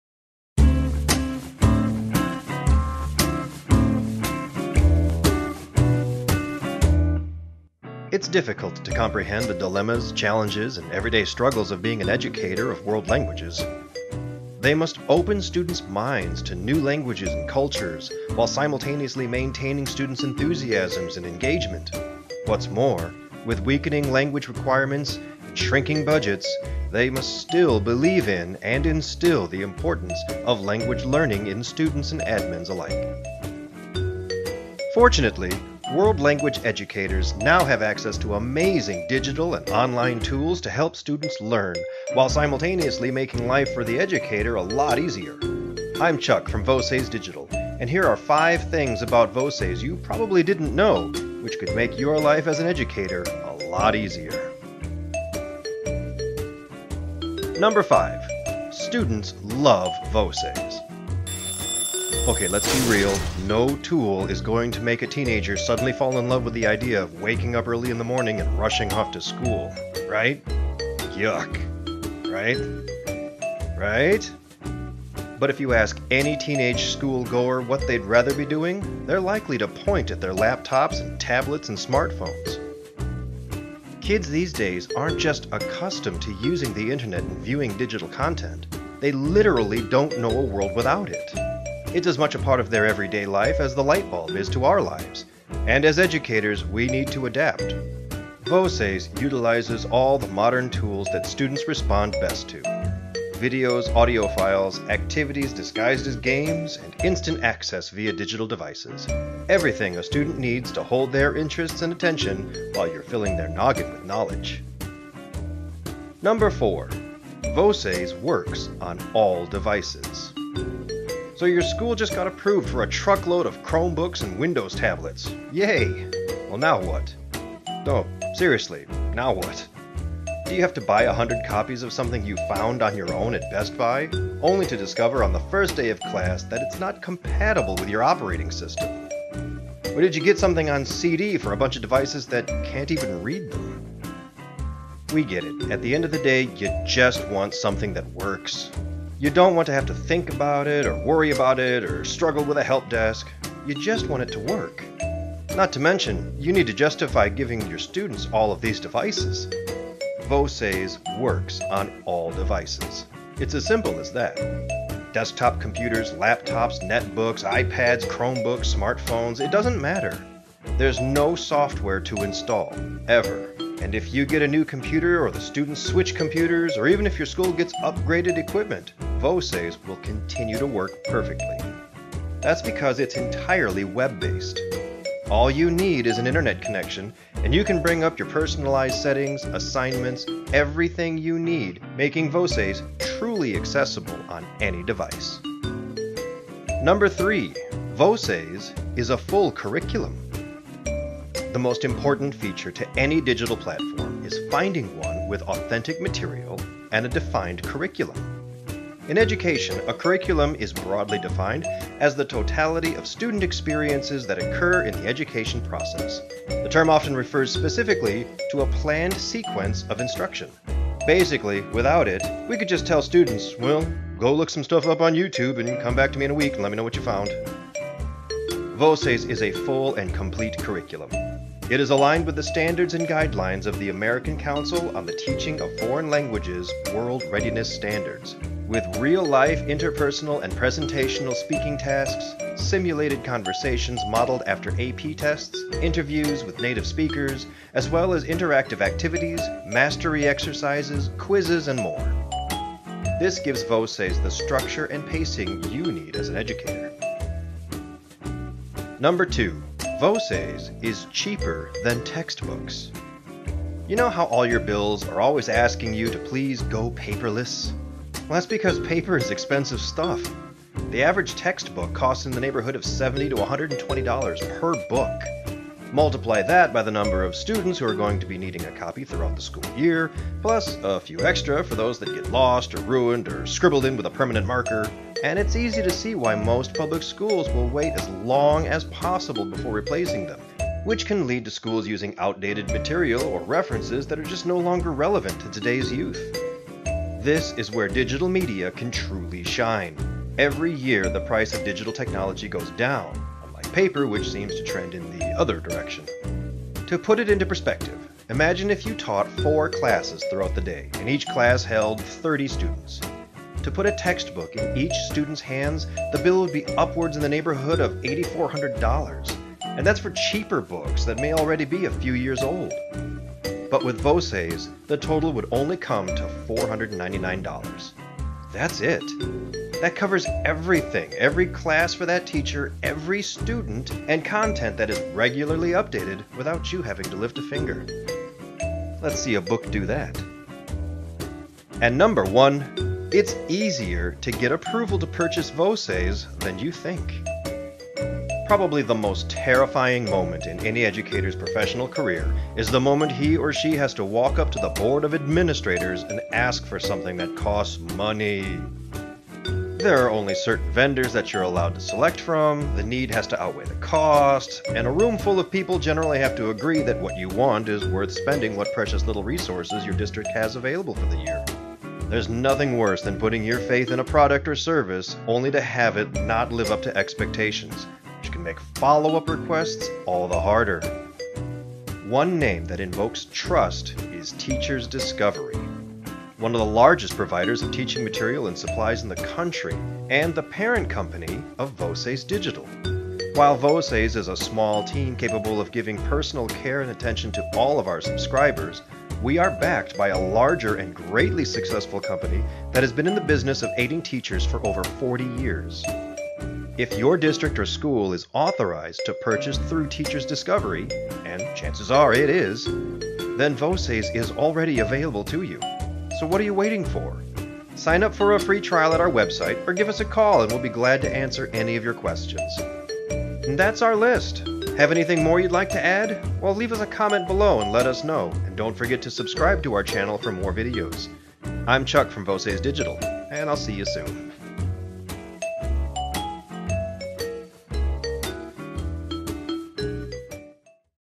5 Things About Voces
Here are "Five Things About Voces Digital" you might not be aware of which could make your life as an educator a lot easier.  Counting down, they are:

5:  Students Love Voces
4:  Voces Works on All Devices
3:  Voces is a Complete Curriculum
2:  Voces is Cheaper Than Textbooks
1:  Voces is Easy to Purchase